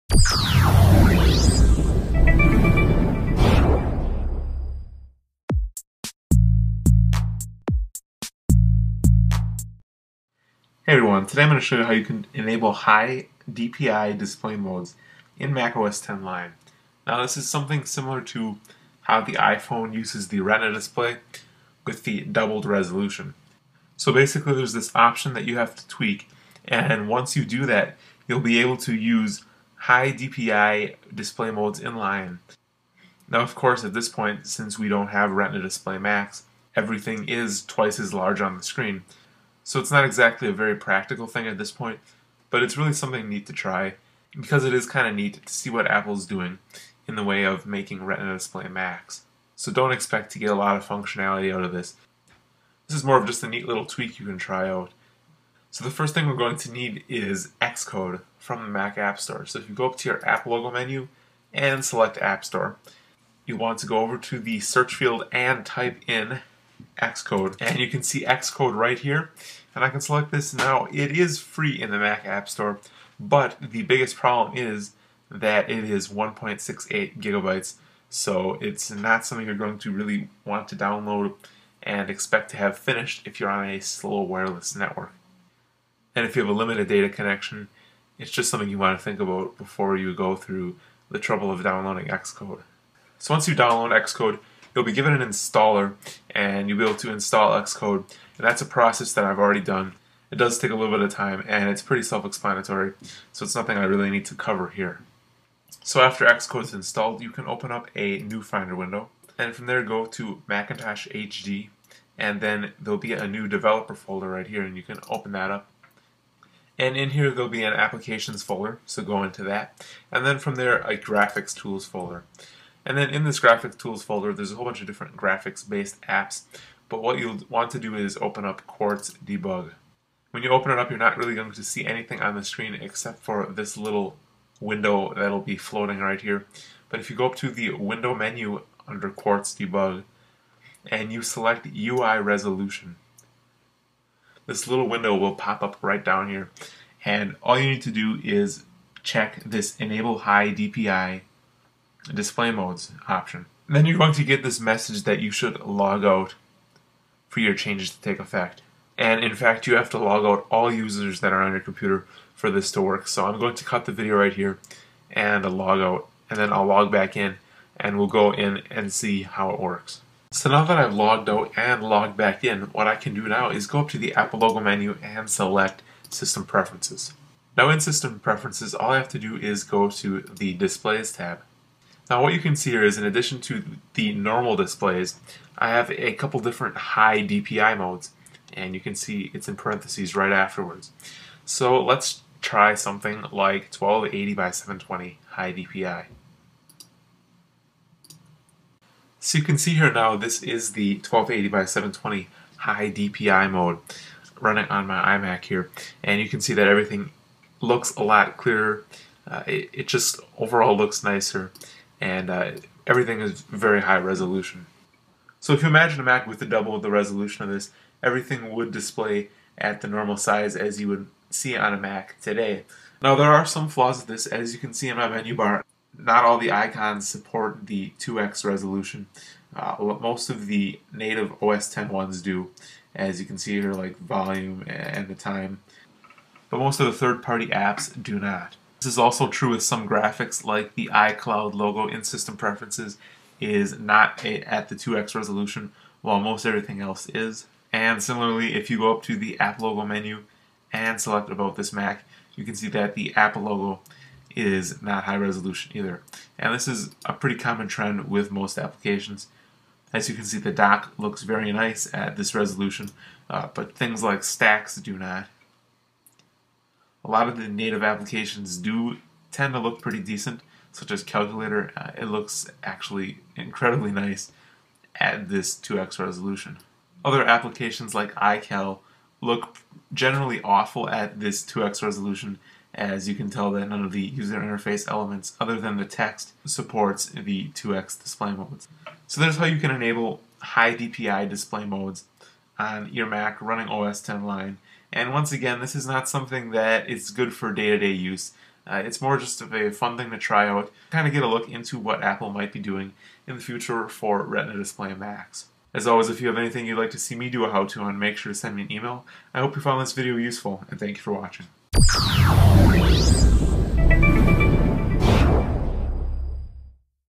Enable HiDPI Display Modes In OS X Lion
A how-to on enabling HIDPI modes in Lion.